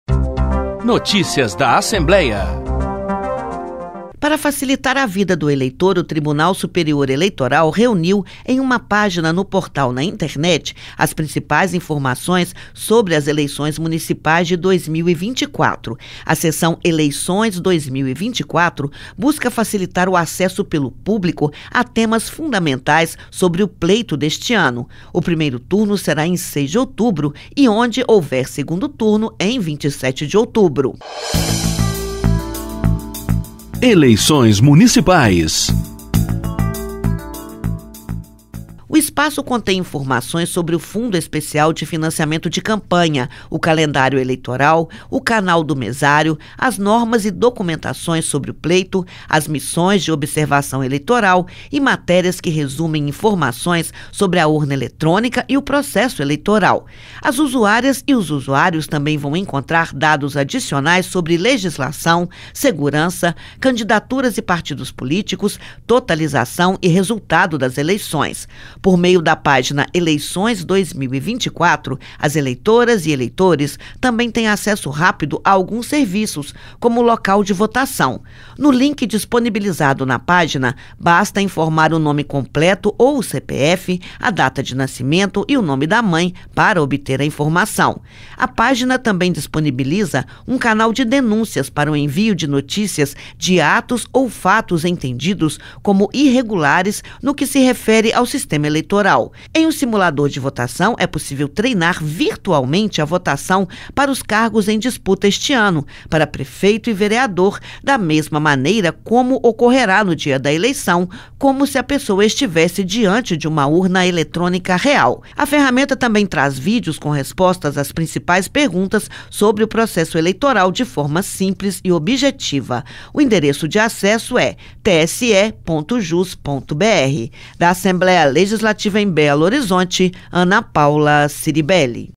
Eleições 2024: Conheça a página que reúne informações essenciais sobre o pleito
Espaço virtual é um atalho para tudo o que o cidadão precisa saber sobre a votação. Acesse